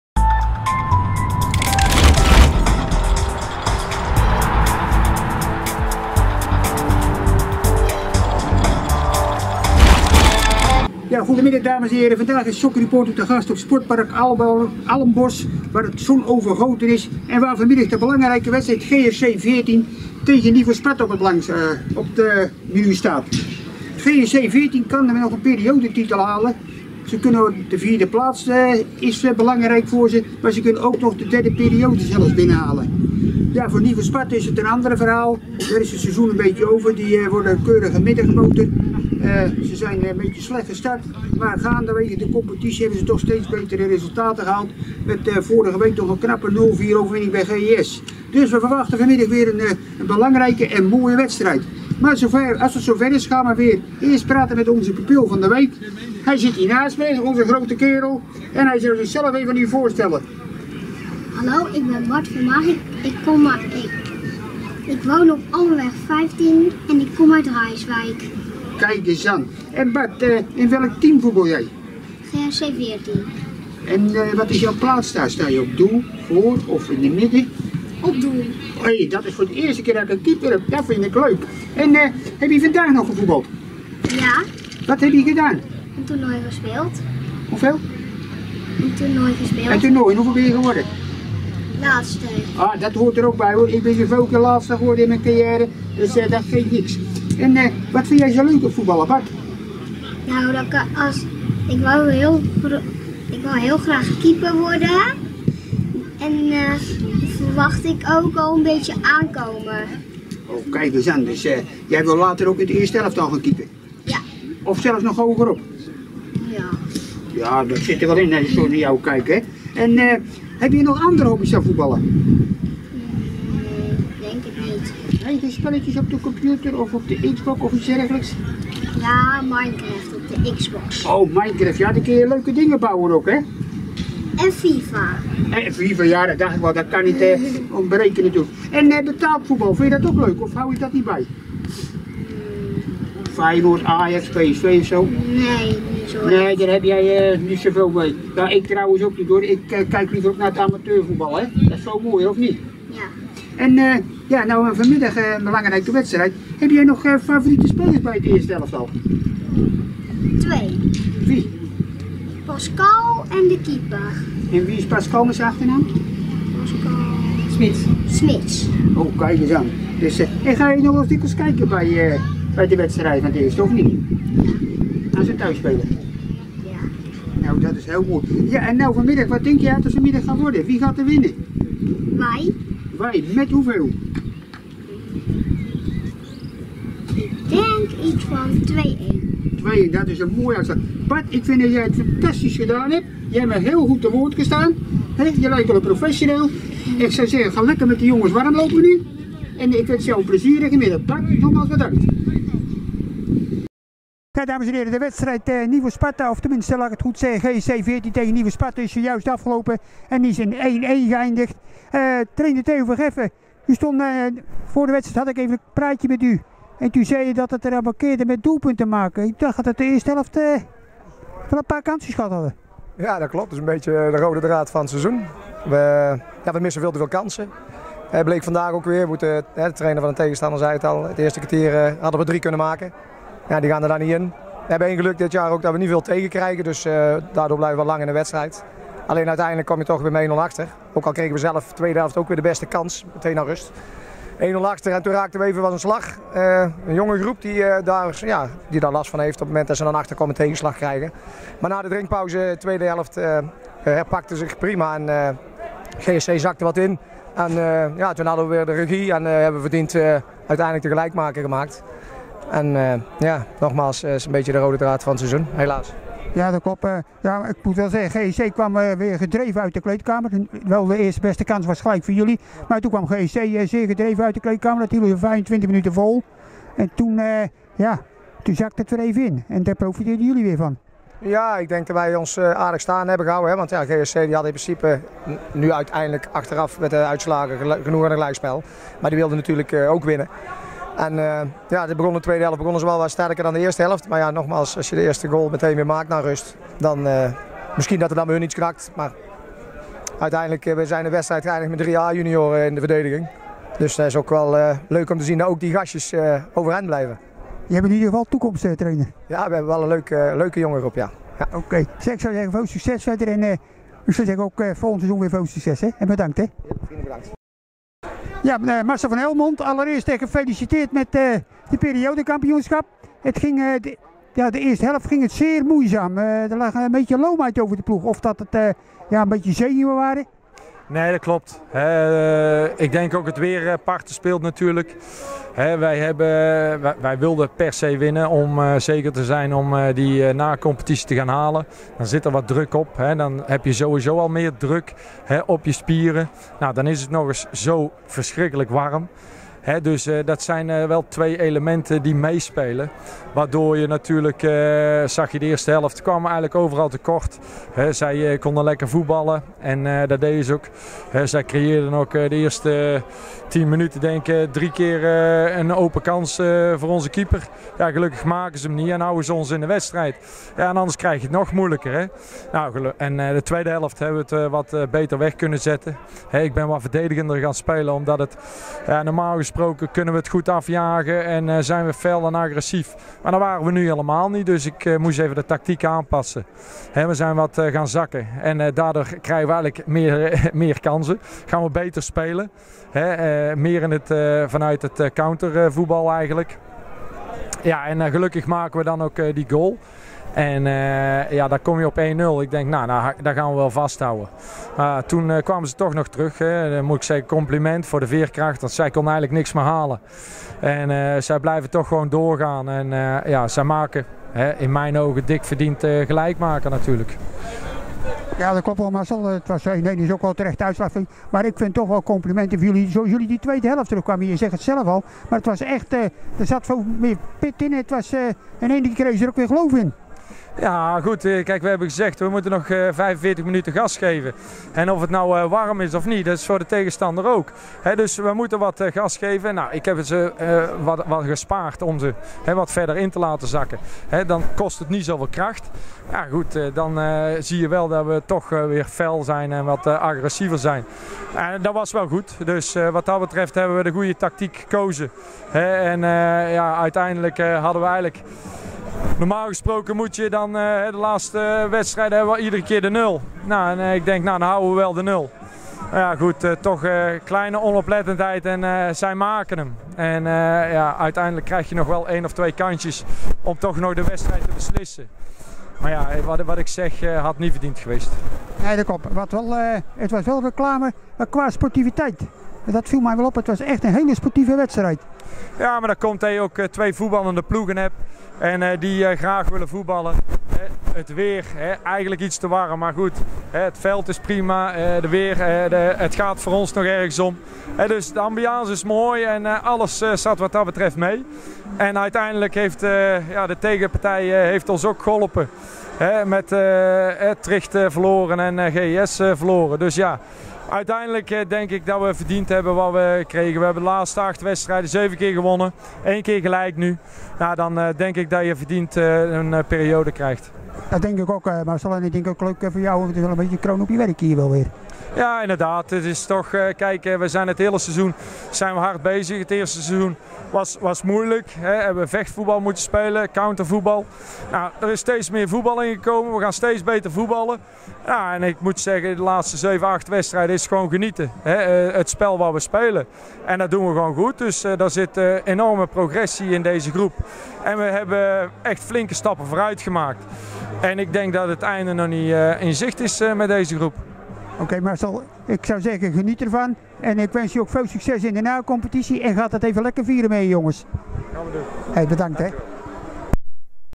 Interviews GRC 14 - Nivo Sparta
Interviews met pupil van de week Bart van Maaren en trainers
Theo van Geffen en Marcel van Helmond